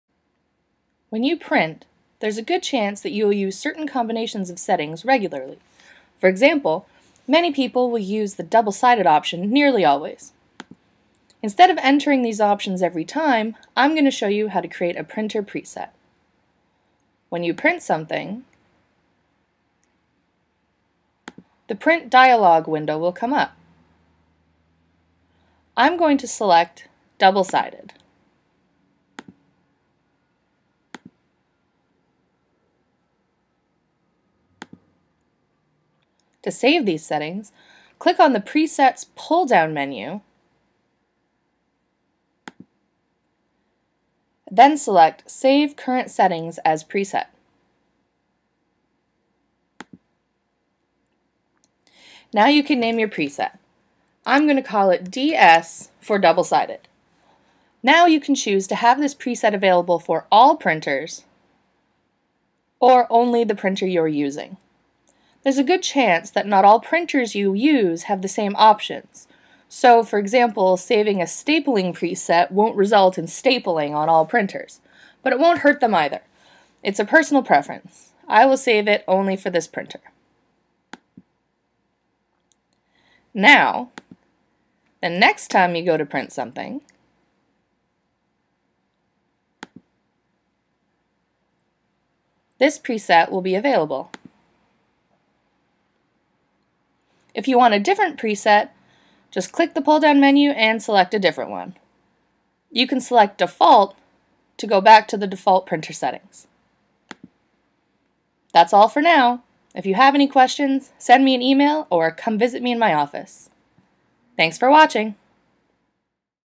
Printer Presets
In this video, you will learn how to set printer presets, which allow you to save frequently used printer settings.